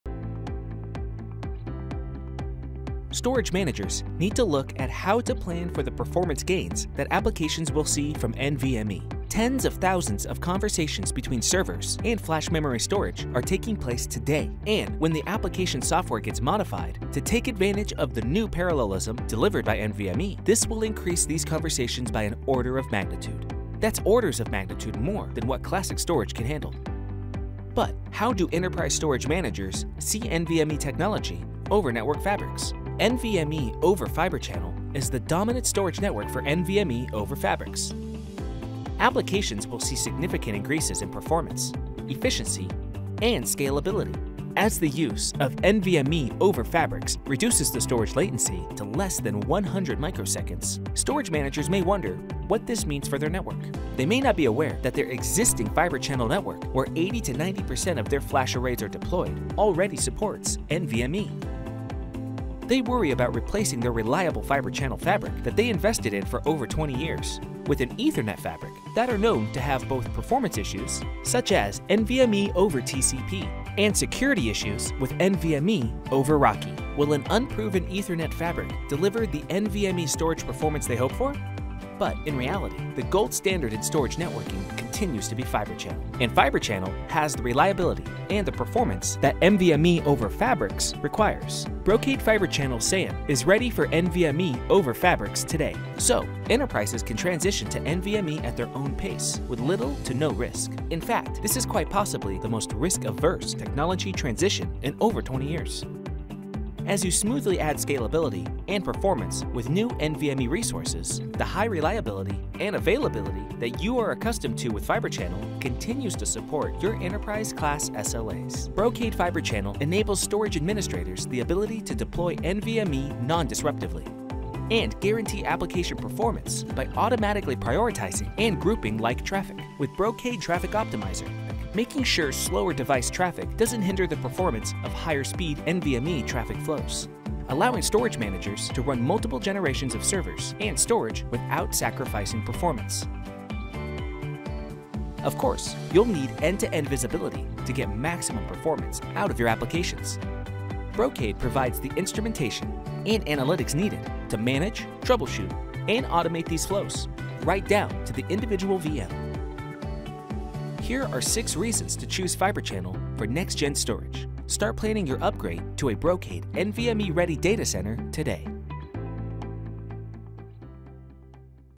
Why NVMe over Fibre Channel
When applications get modified to take advantage of the performance delivered by NVMe, you need to consider how this will place increased demands on the storage network, making it important to plan for NVMe over Fabric today. It is important to understand the pros and cons of both NVMe over Fibre Channel vs TCP/IP, and how they can impact your application performance, scalability, reliability and more. Learn how easy it is to take advantage of the latest performance improvements in NVMe storage using your NVMe-ready Fibre Channel storage network without compromising on the high reliability and availability necessary to support your enterprise class SLAs.